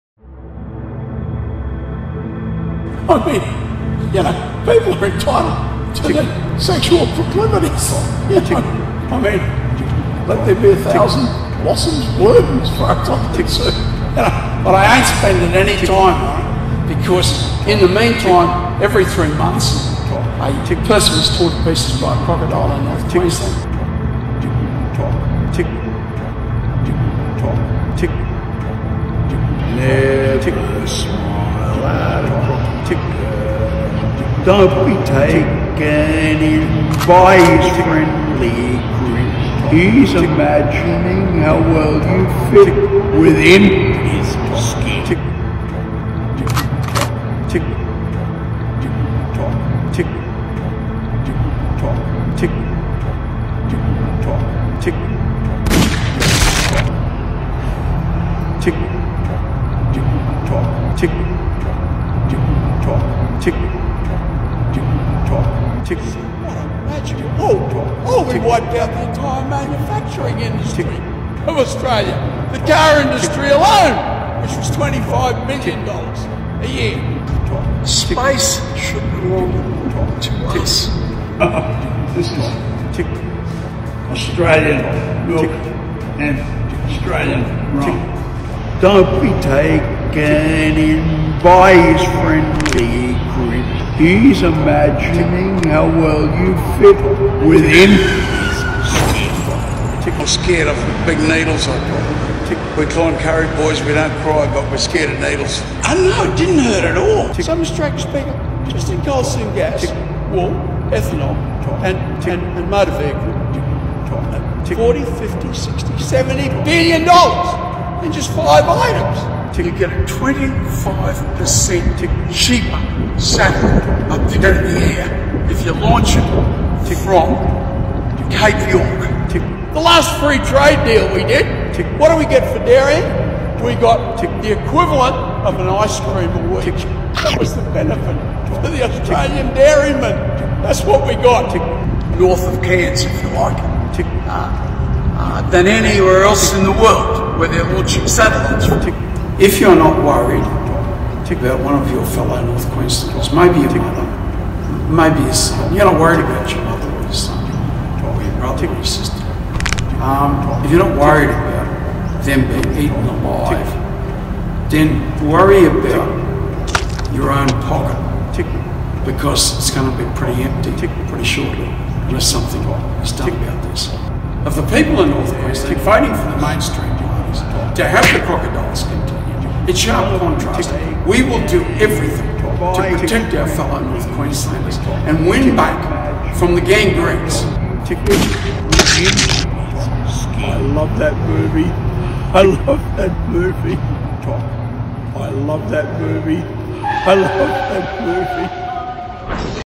Bob Katter joins TikTok to destroy all crocodiles
Bob Katter has taken his revenge mission against his enemy, the crocodiles, online and has finally found a way to rid them once and for all.